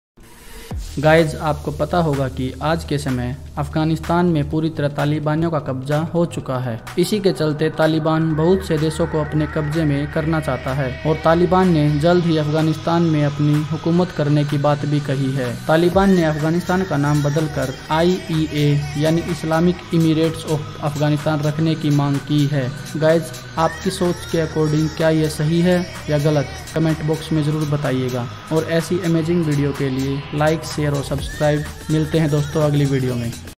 Afganistan ka Name bdla gya#A2motivation
Pentagon officials believe the attack at Kabul airport on Thursday was a result of one explosion, not two.

“We do not believe that there was a second explosion at or near the Baron hotel, that it was one suicide bomber,” US Army Major General William Taylor told reporters at the Pentagon in Washington, DC.

KEEP READING
In Pictures: Deadly explosions outside Kabul airport
Kabul bombings: 13 US army personnel killed in attacks at airport
Kabul bombings: ‘We will hunt you down,’ Biden warns attackers
At least 110 people have been killed in the explosion outside Kabul airport, according to Al Jazeera’s team in Afghanistan.

The US troops helping to evacuate Afghans desperate to flee Taliban rule are bracing for more attacks was a result of only one, not two explosions.

Al Jazeera’s Charles Stratford, reporting from Kabul, said the evacuation has resumed.

“We have seen a number of planes take off. The number of people around the airport has grown dramatically since yesterday,” he said. “We have seen thousands of people scrambling up against a wall there desperate to get on the few remaining planes.”